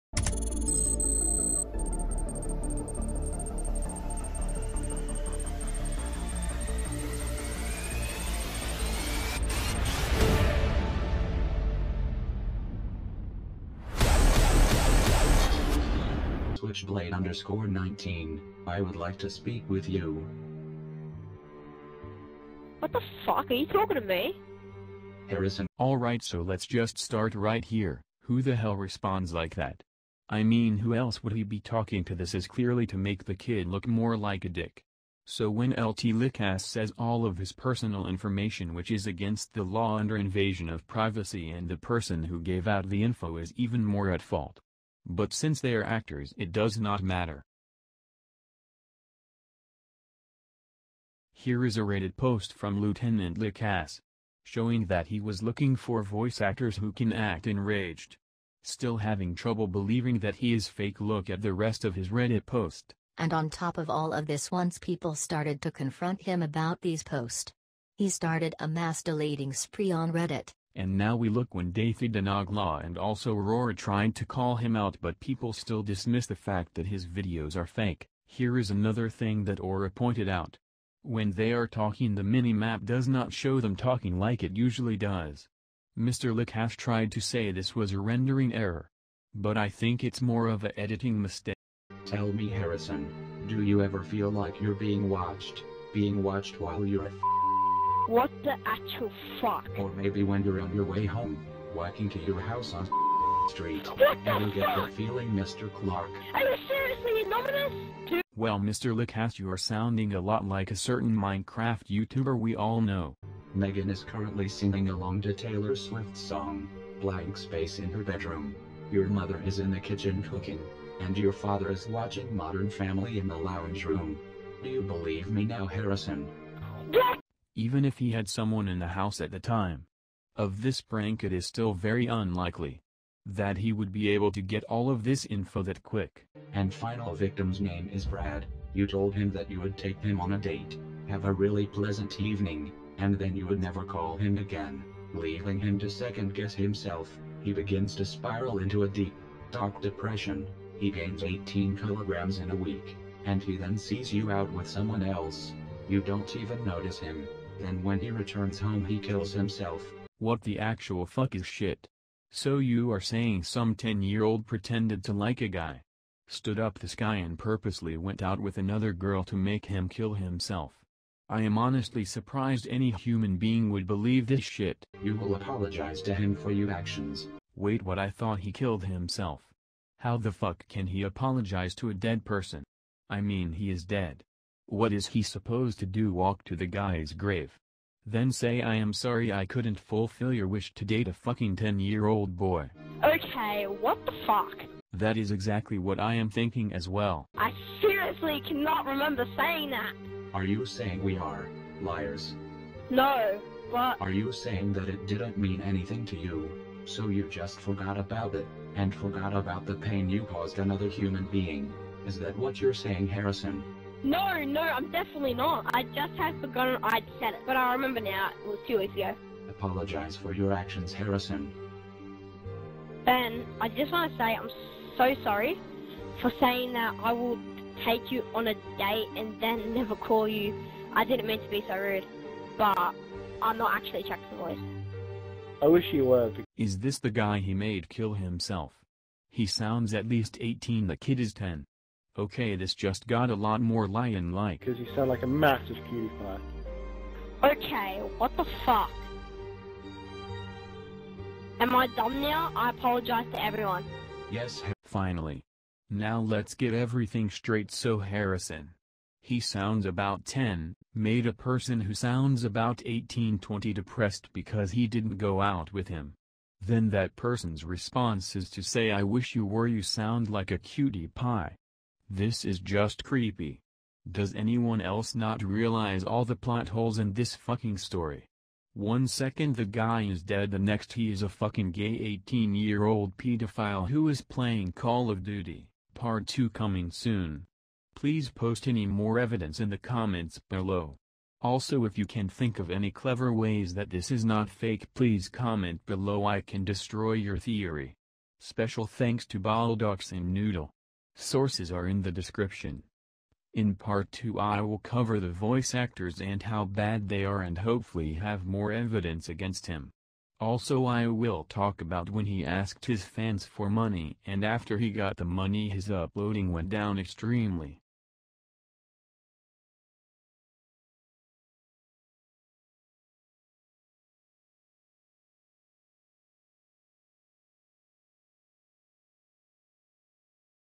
lieutenant lick me EXPOSED FAKE VIDEOS!
I know this is not guarunteed evidence but it is enough evidence to look into it especially since he has gotten so much money from his fans donating to his "sick dad".

Please share this and i give anyone 100% permission to reupload this video just give me some credit.